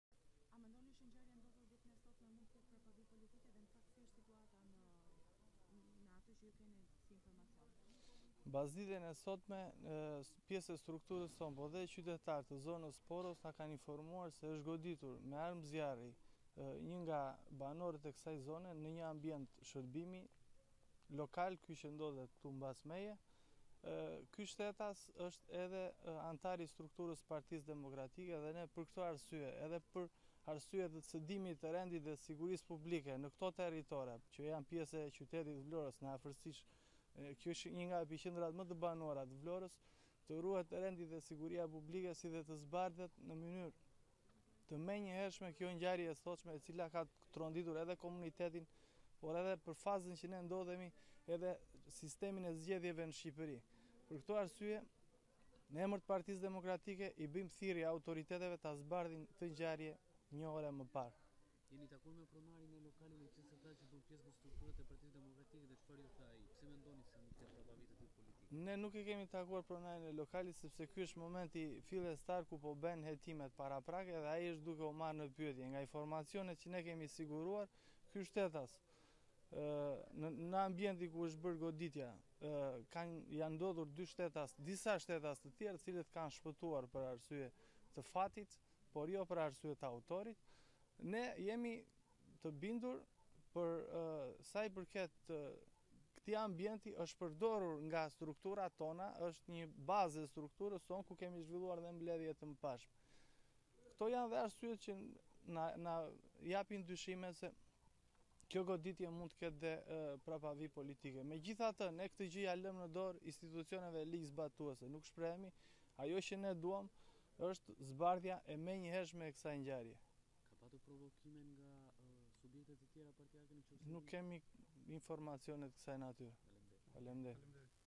21 të shtëna kallashnikovi në lokal, reagon PD: Dyshojmë se goditja ka prapavijë politike
Fshati Poro i Vlorës është tronditur pasditen e sotme nga 21 të shtënat e kallashnikovit në drejtim të një lokali, i cili shërbente edhe si bazë për strukturat e Partisë Demokratike. Në një deklaratë për mediat zëvendës kryetari i PD-së në Vlorë Lorenc Danaj, u shpreh se pronari i lokalit është anëtar i PD-së. Ai i bëri thirrje për ruajtje të rendit dhe sigurisë publike në prag të zgjedhjeve dhe kërkoi zbardhjen e menjëhershme të ngjarjes.